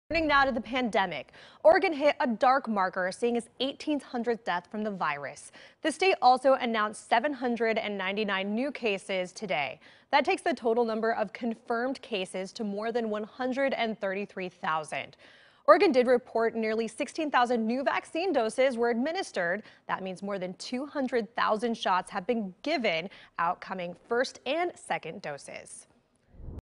Oregon COVID-19 updated Jan. 17, 2021
Oregon recorded its 1,800th COVID-19 death on Jan. 17, 2021.